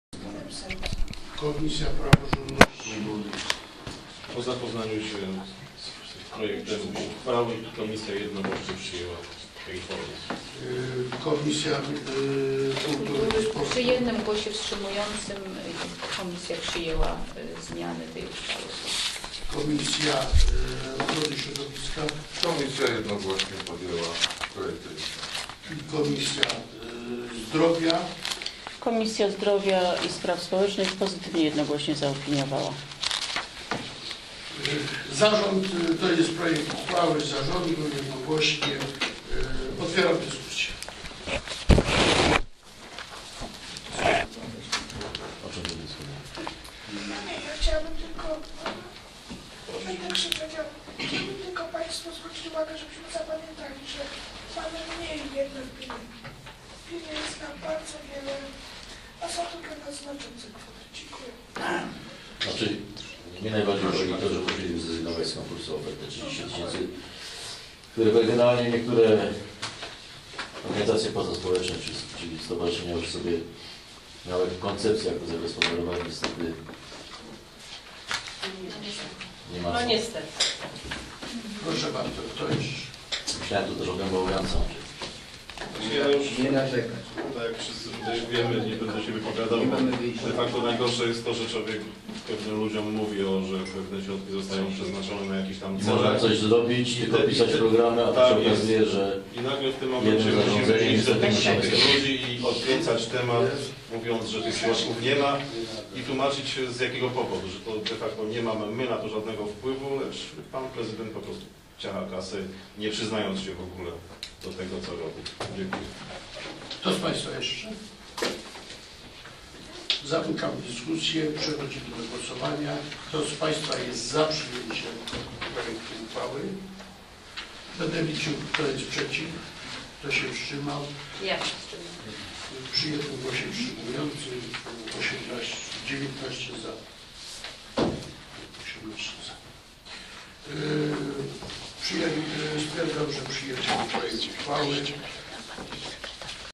RD_XVII_27-09-2012_1 korekta środków na zadania priorytetowe w roku 2013
Obrady XXII sesji Rady Dzielnicy XVII Wzgórza Krzesławickie z dn. 27 września 2012.
Korekta zadań i podziału środków przeznaczonych na przedsięwzięcia priorytetowe Dzielnicy XVII w roku 2013.